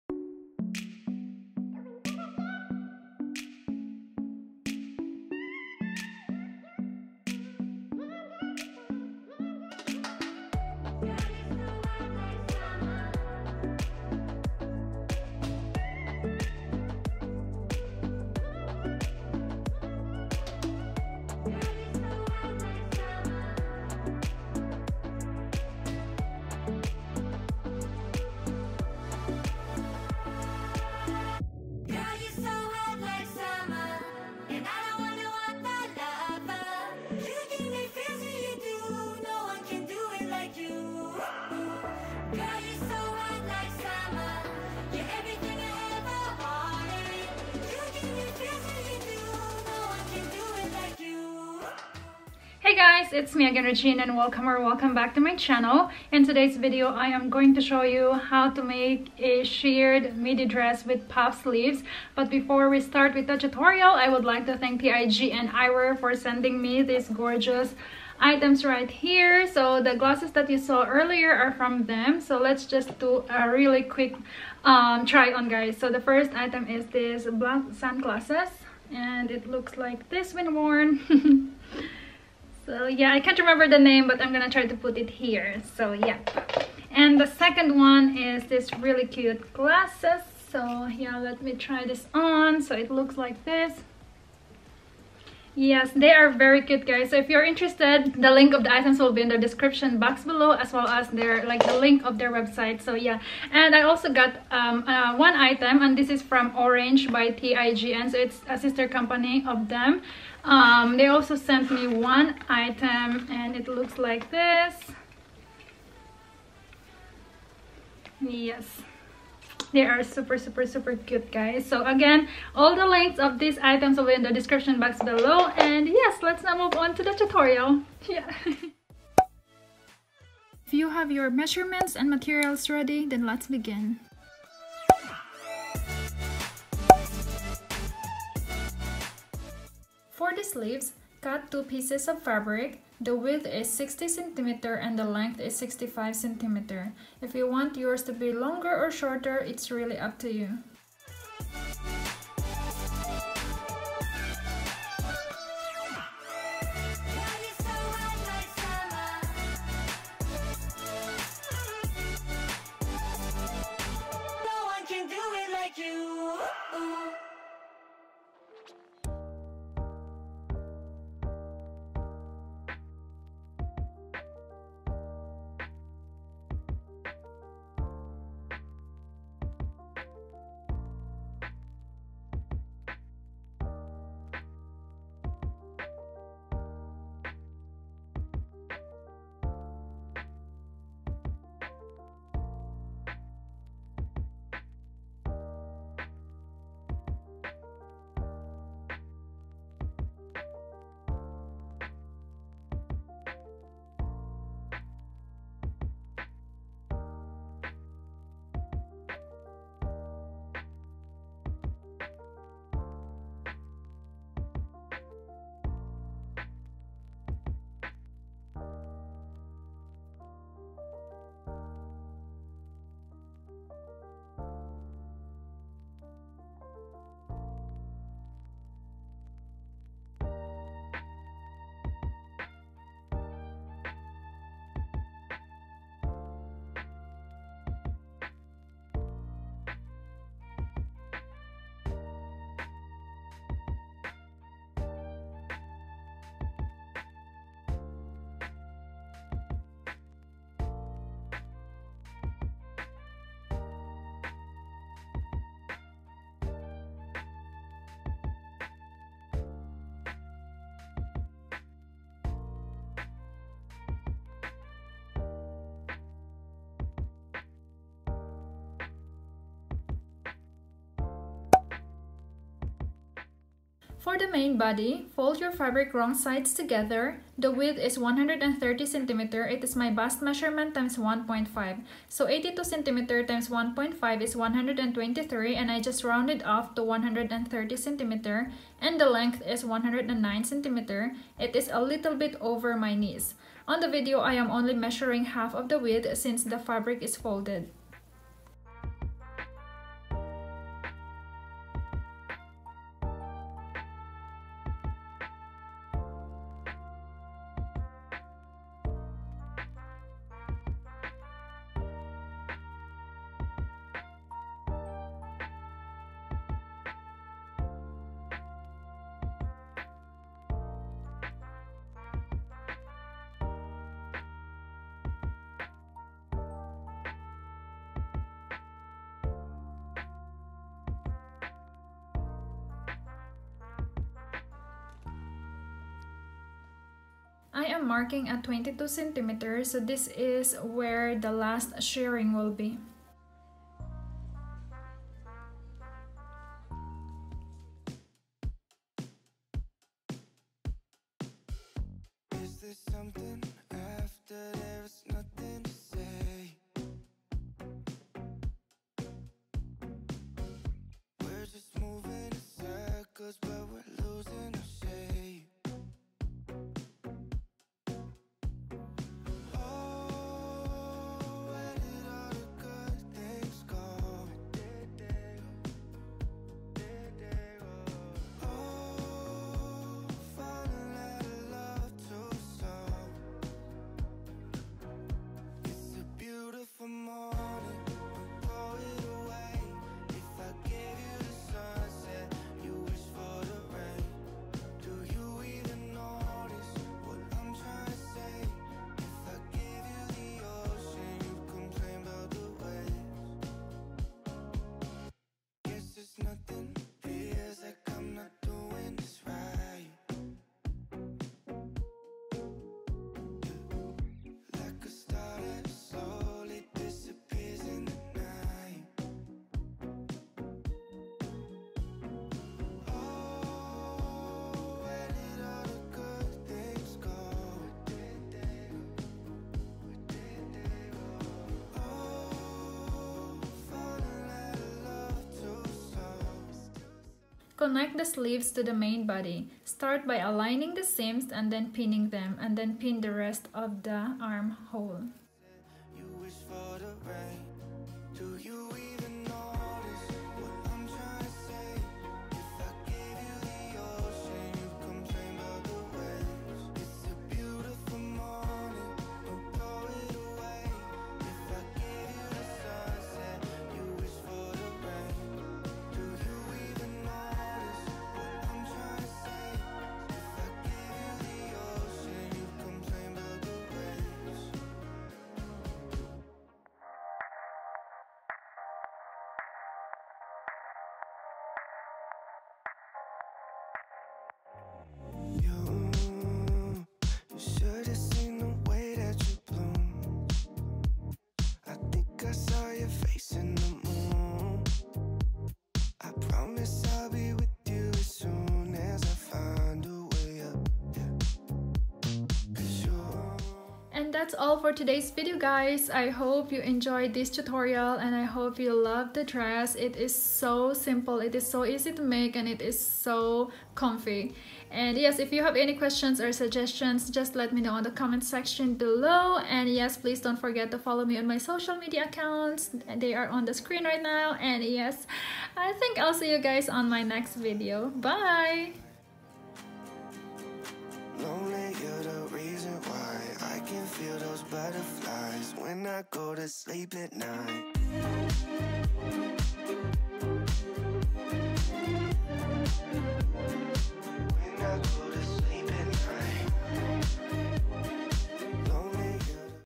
DIY SHIRRED MIDI DRESS WITH PUFF SLEEVES ft. TIJN EYEWEAR | Regine Morales
Hi everyone!! I miss you so much. hahaha

I am back with another diy video. In this video I am showing you how to make a shirred midi dress with puff sleeve which is so perfect for summer. Enjoy everyone.

YOU WILL NOT KNOW IF YOU WILL NOT TRY!